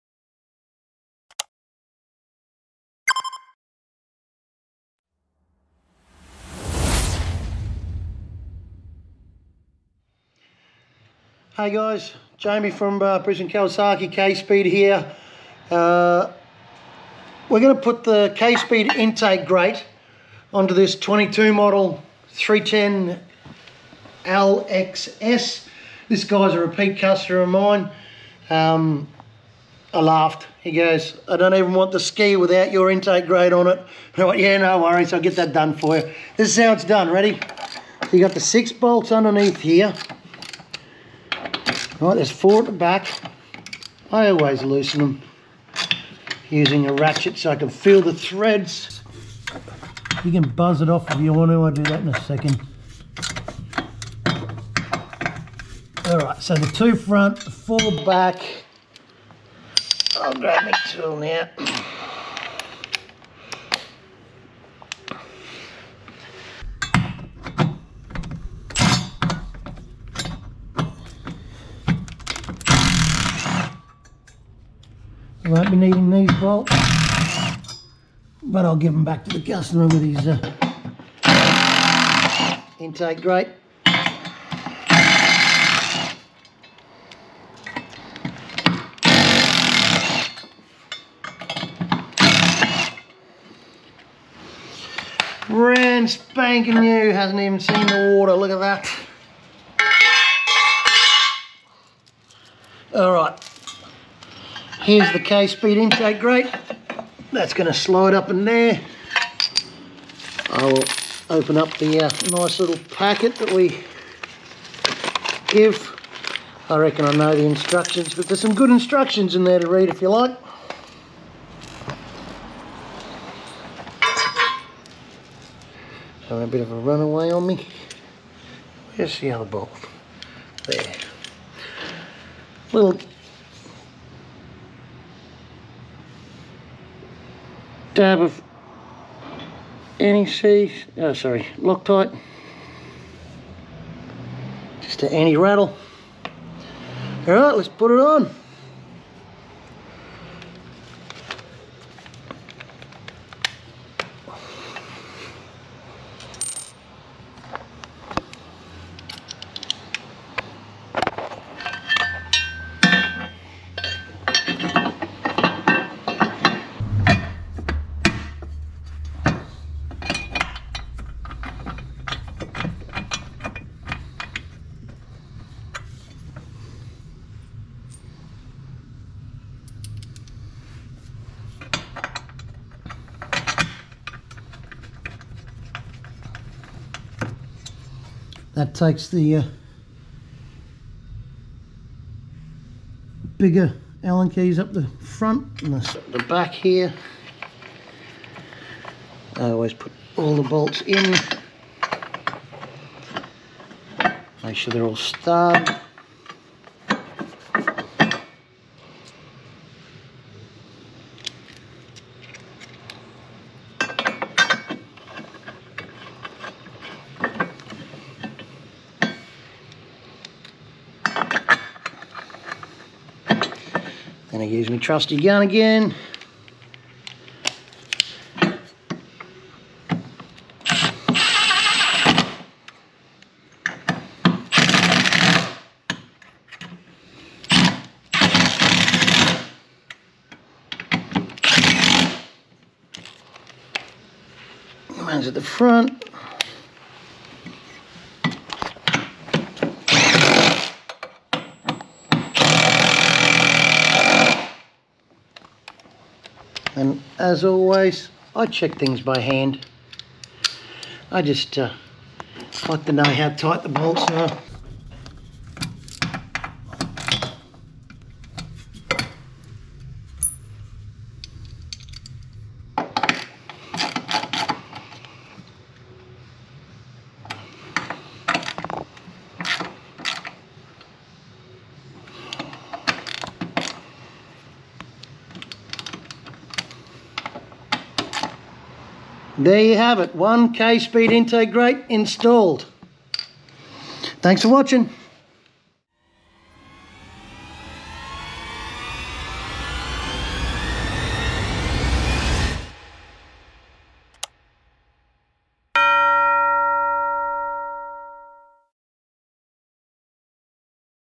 Kawasaki Ultra 310 - Kspeed Intake Grate Install
We install a Kspeed Intake Grate on this brand new, zero hour 2022 Kawasaki Ultra 310 LX-S.

This video works for the Kawasaki Ultra 300, 310 and the new 2022 310.